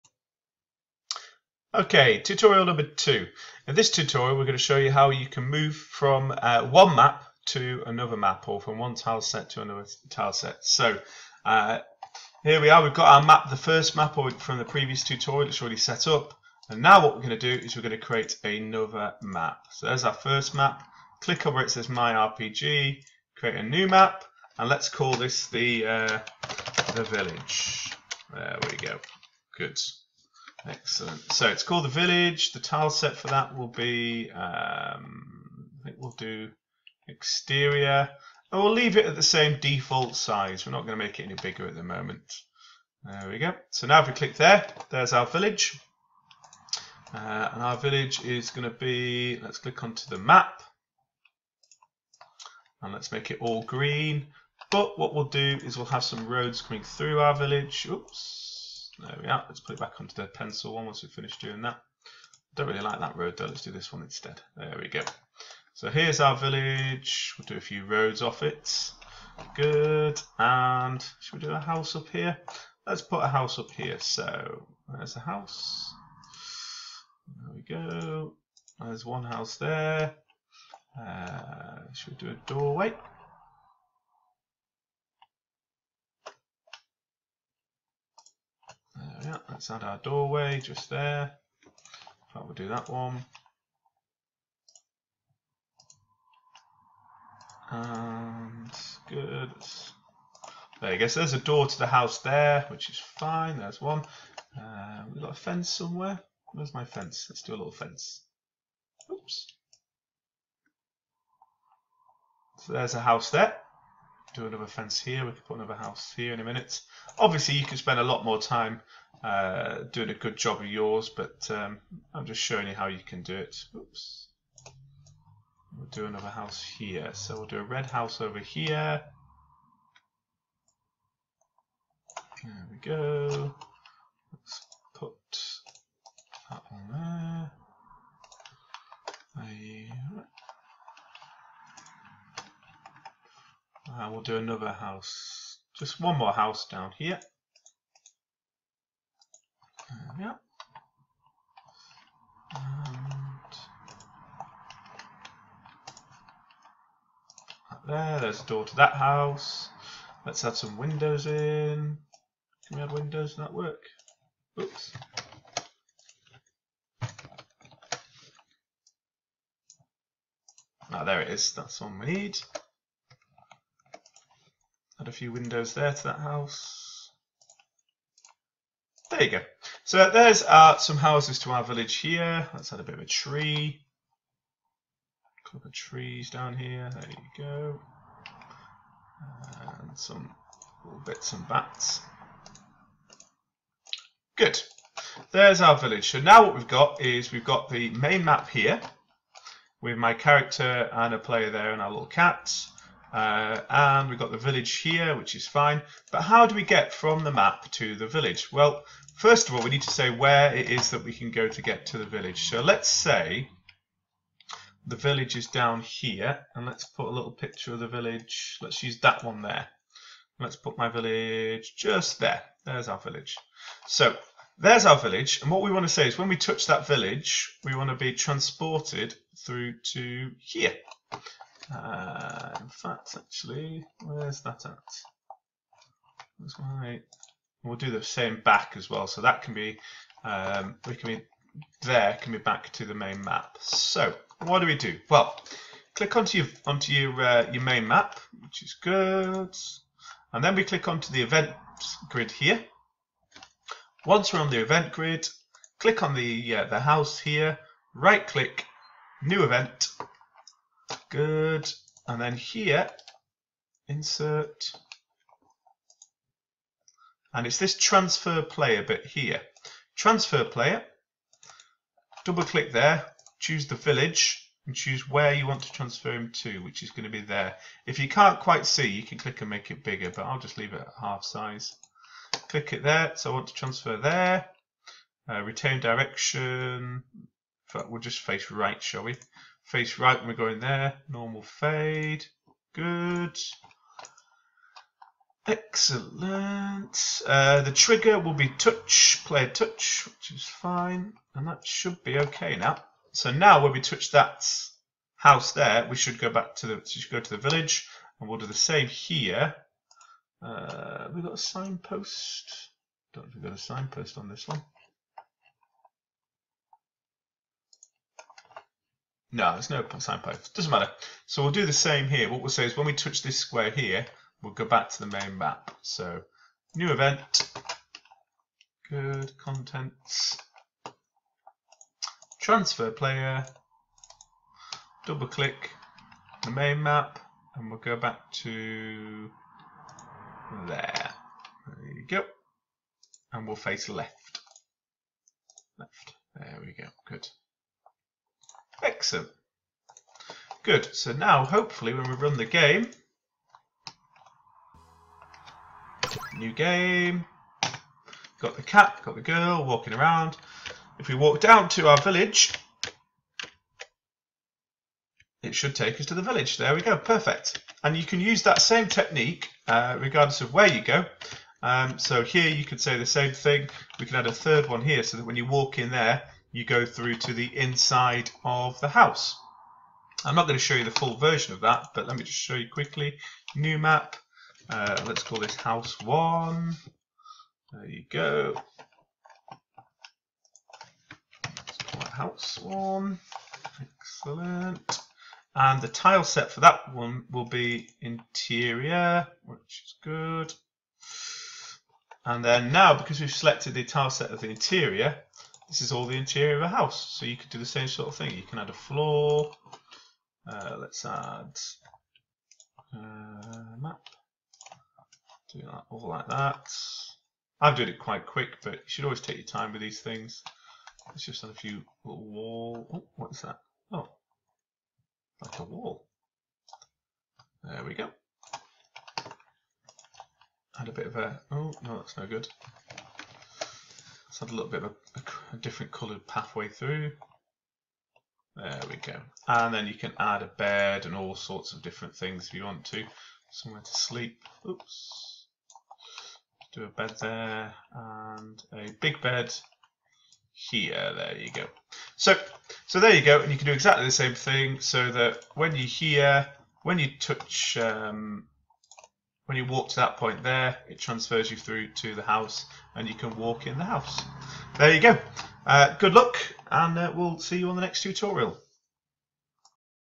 RPG Maker VX Ace Tutorial 2 Moving from map to map
How to allow the player to move from map to map, so that you can create multiple map games. Also works for warping a player from one area of the screen to another, like using a cave or similar.